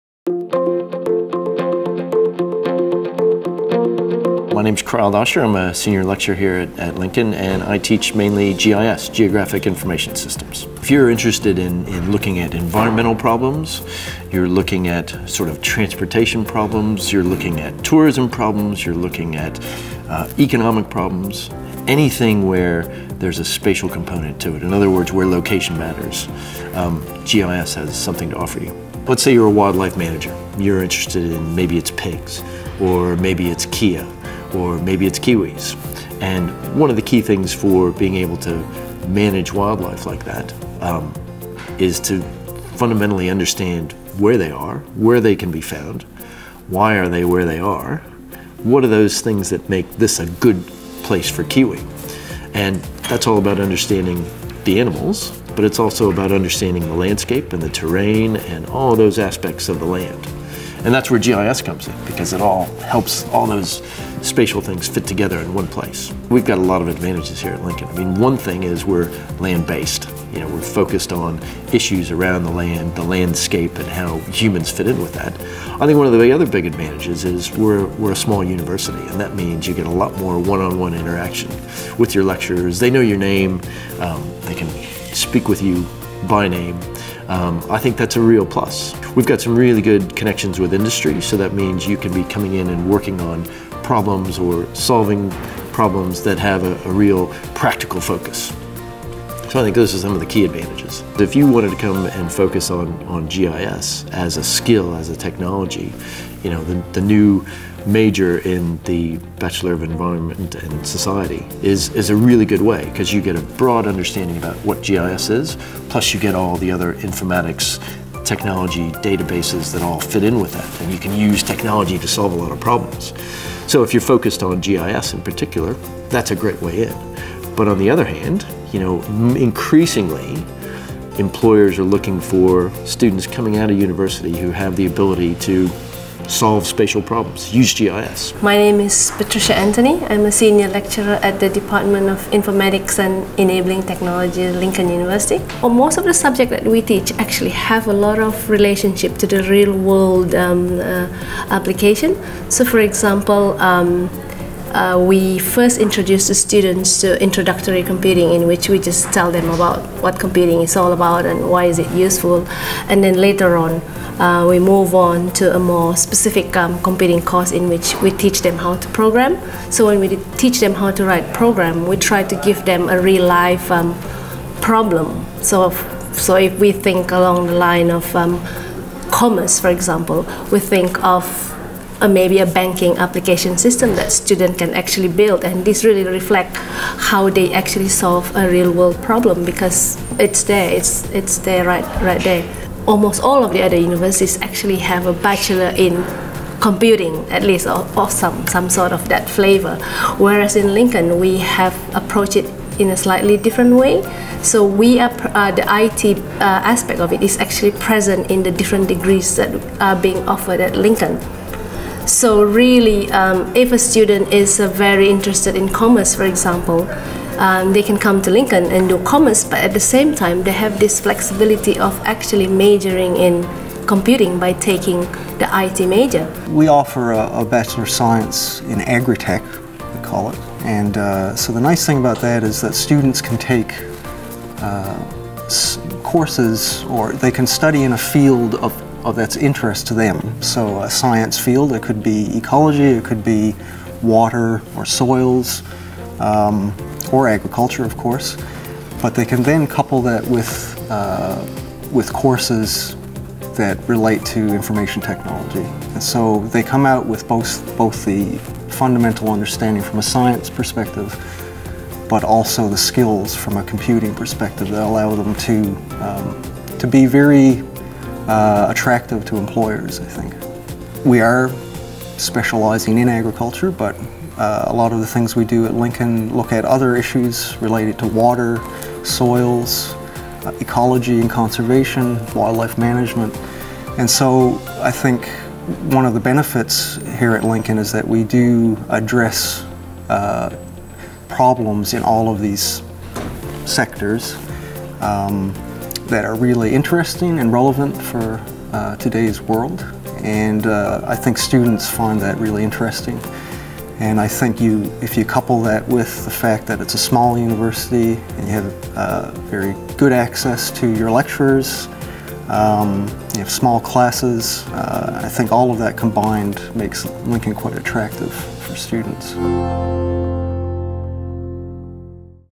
Study Information Technology at Lincoln
The information technology (IT) industry is constantly changing. The opportunities and career prospects are high, along with the potential for new discoveries with scientific and commercial value.

So if you love IT, great!  Lincoln is the IT place to be – come and visit us to learn about the vast and differing places information technology can take you.